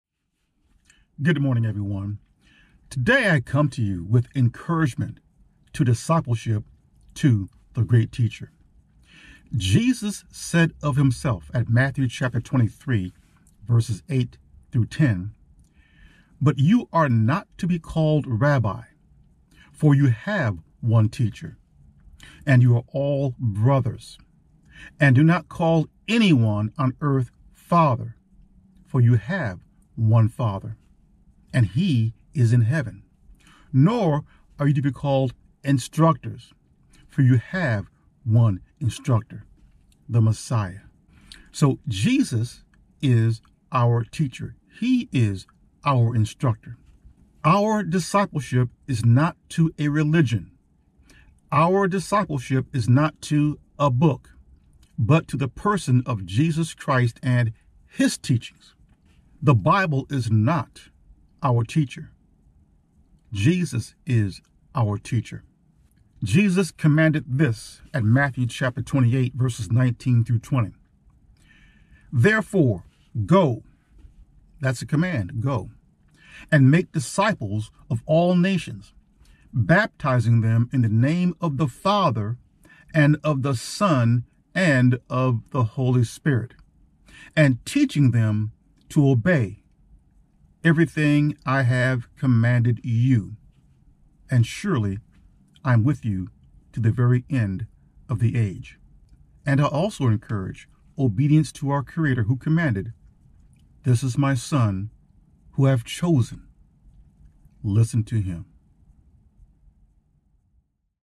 Encouraging Discipleship To The Great Teacher
In this short video, I would like to encourage "Discipleship" (becoming a student-follower) of The Greatest Teacher To Have Ever Lived, God's Only Begotten Son, The Messiah Jesus.